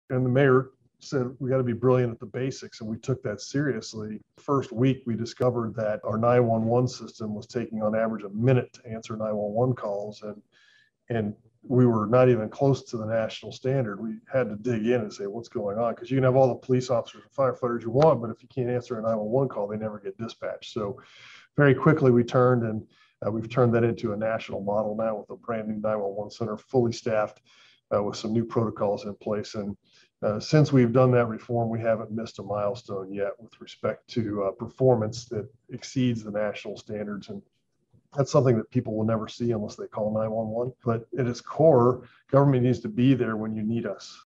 Being Brilliant At The Basics - Doug McGowen | Ask Alan! The Podcast Highlights
With Doug McGowen becoming the new CEO of MLGW, we look back to when he was on Ask Alan! The Podcast discussing how he was able to solve a problem for our city. Looking forward to seeing what he can accomplish at MLGW!

For more Ask Alan! The Podcast, click right here!

The Crone Law Firm, PLC
88 Union Ave., 14th Floor
Memphis, Tennessee 38103